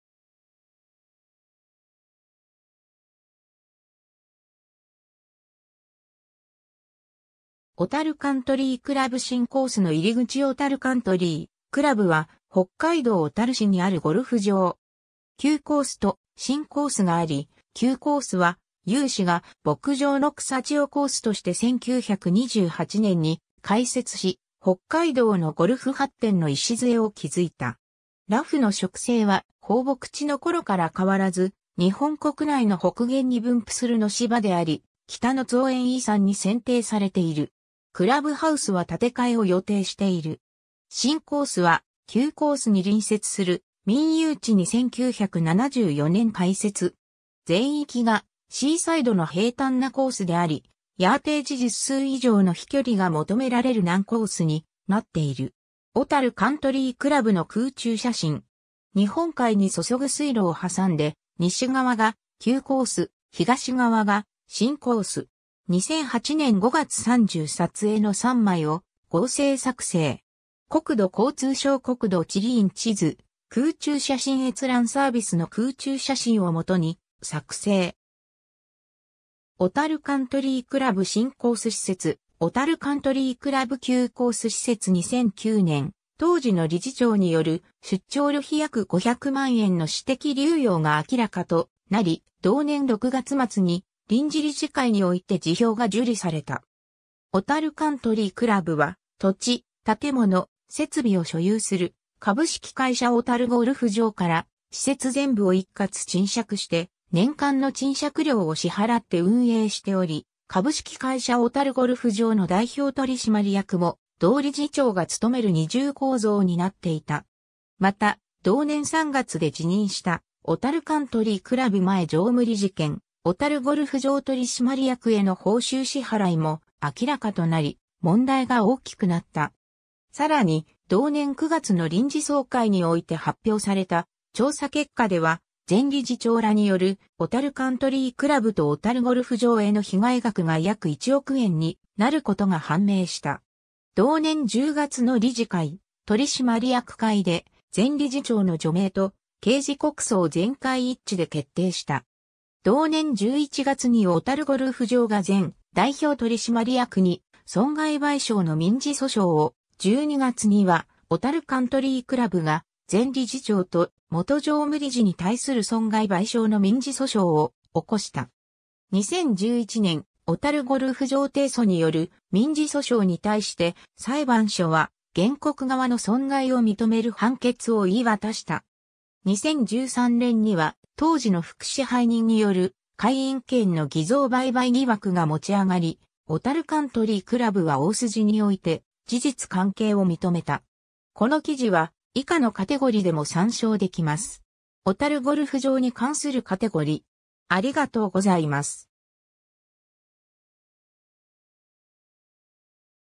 小樽カントリー倶楽部
小樽カントリー倶楽部新コースの入口 小樽カントリー倶楽部（おたるカントリーくらぶ）は、北海道小樽市にあるゴルフ場。
 旧コースと新コースがあり、旧コースは有志が牧場の草地をコースとして1928年（昭和3年）に開設し、北海道のゴルフ発展の礎を築いた。
ラフの植生は放牧地の頃から変わらず、日本国内の北限に分布するノシバであり、「北の造園遺産」に選定されている。
クラブハウスは建て替えを予定している。
 新コースは旧コースに隣接する民有地に1974年（昭和49年）開設。
全域がシーサイドの平坦なコースであり、ヤーテージ実数以上の飛距離が求められる難コースになっている。
 小樽カントリークラブの空中写真。
日本海に注ぐ水路を挟んで西側（左）が旧コース、東側（右）が新コース。
 2008年5月30撮影の3枚を合成作成。
 小樽カントリー倶楽部 新コース 施設 小樽カントリー倶楽部 旧コース 施設 2009年（平成21年）、当時の理事長による出張旅費約500万円の私的流用が明らかとなり、同年6月末に臨時理事会において辞表が受理された。
小樽カントリー倶楽部は、土地・建物・設備を所有する株式会社小樽ゴルフ場から施設全部を一括賃借して年間の賃借料を支払って運営しており、株式会社小樽ゴルフ場の代表取締役も同理事長が務める二重構造になっていた。
また、同年3月で辞任した小樽カントリー倶楽部前常務理事兼小樽ゴルフ場取締役への報酬支払いも明らかとなり（小樽ゴルフ場の定款では、取締役及び監査役の報酬は全員無報酬としており、小樽カントリー倶楽部の役員も名誉職とされ、職務のために要した実費しか請求出来ない）、問題が大きくなった。
さらに、同年9月の臨時総会において発表された調査結果では、前理事長らによる小樽カントリー倶楽部と小樽ゴルフ場への被害額が約1億円になることが判明した。
同年10月の理事会・取締役会で前理事長の除名と刑事告訴を全会一致で決定した。
同年11月に小樽ゴルフ場が前代表取締役に損害賠償の民事訴訟を、12月には小樽カントリー倶楽部が前理事長と元常務理事に対する損害賠償の民事訴訟を起こした。
2011年（平成23年）、小樽ゴルフ場提訴による民事訴訟に対して、裁判所は原告側の損害を認める判決を言い渡した。
 2013年（平成25年）には当時の副支配人による会員権の偽造売買疑惑が持ち上がり、小樽カントリー倶楽部は大筋において事実関係を認めた。
 この記事は以下のカテゴリでも参照できます。
 小樽ゴルフ場に関するカテゴリ: